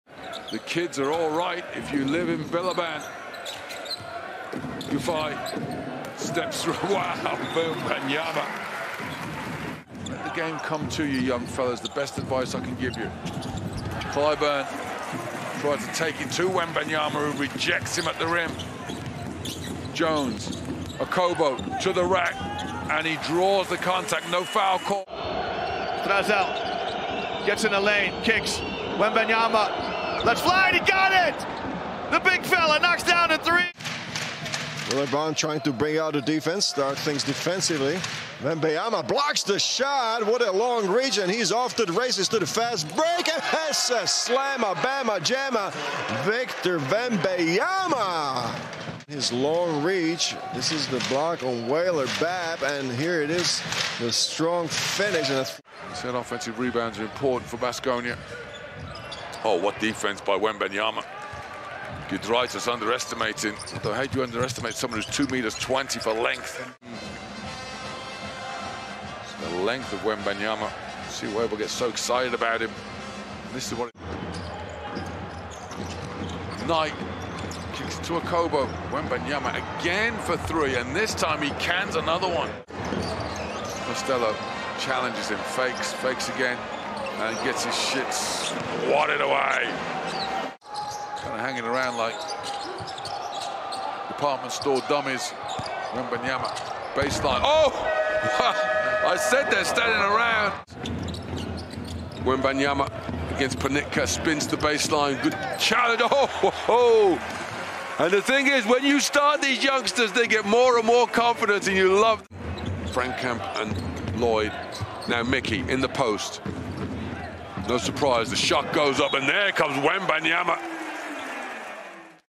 Victor Wembanyama, The French Unicorn
In recent years, basketball fans have a nickname for players with unique talent: unicorn. Few players fit into this category but LDLC ASVEL Villeurbanne Rising Star Victor Wembanyama is definitely one of them.